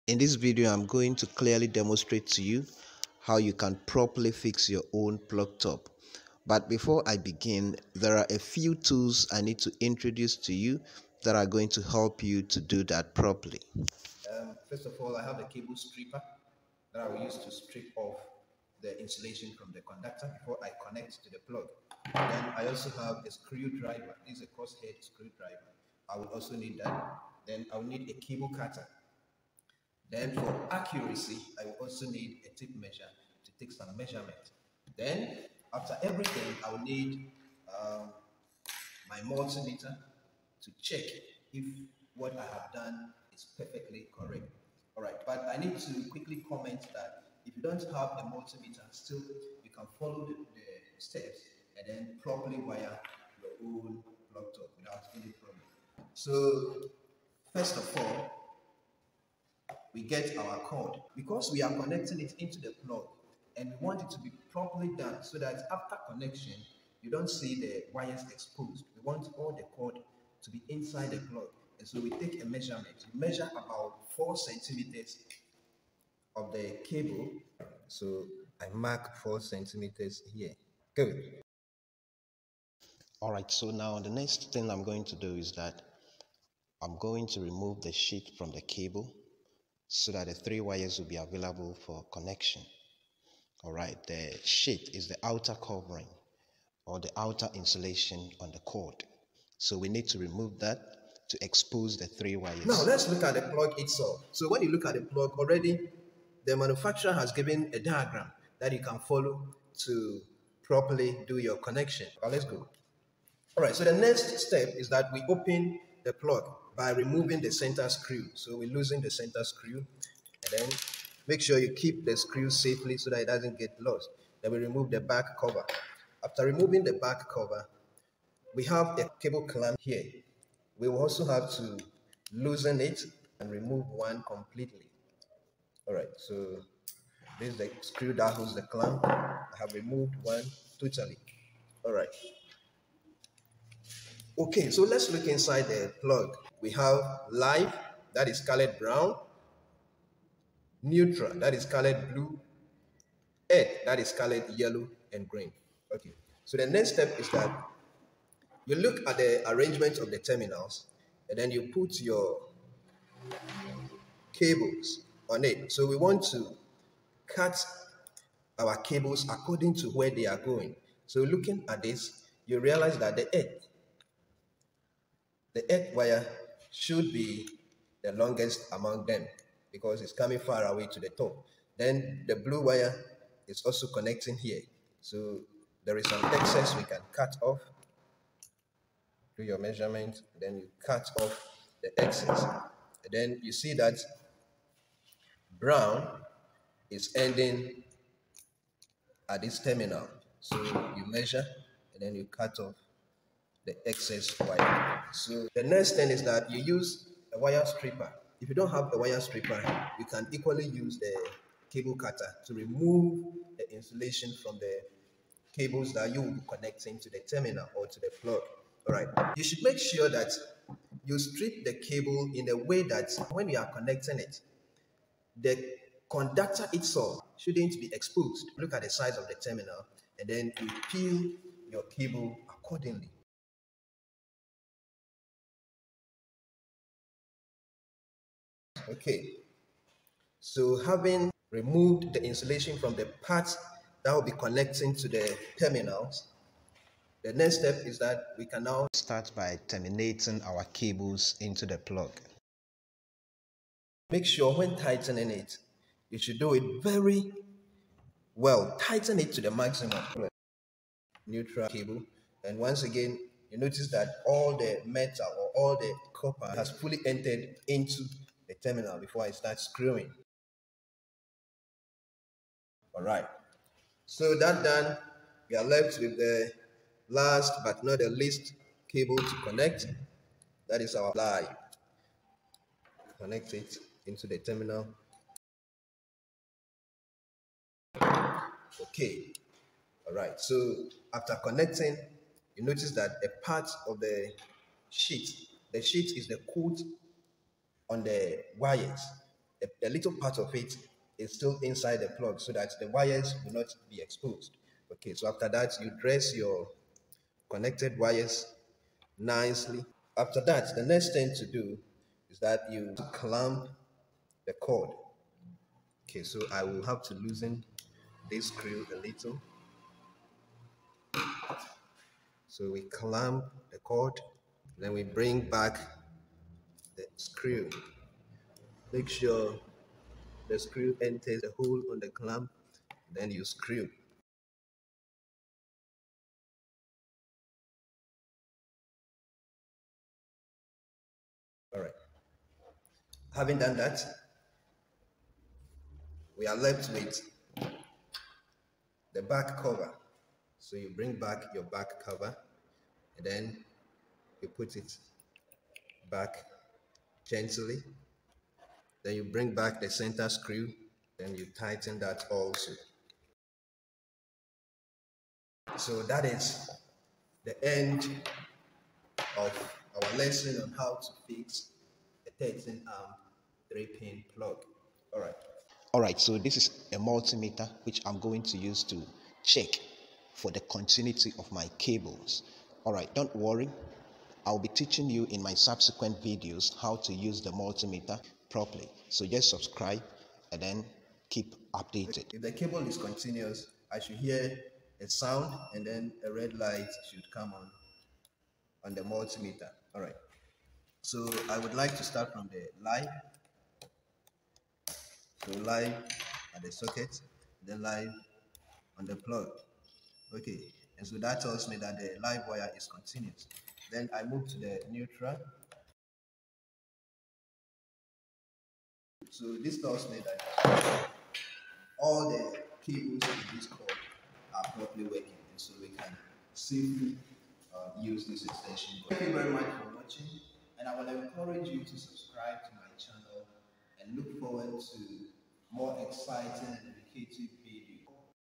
HOW TO PROPERLY WIRE A PLUG
This video demonstrates how to properly wire a plug.
The three pin plug needs all  three wires to connect to it properly for safty.
the cables used in the video has the new wiring colour code. to know more about wiring colour codes.

contuinity test was done to check the contuinity of all the wires connected to the plug
I hope this video will be helpful to anyone including electrical students, who would want to connect their own plug.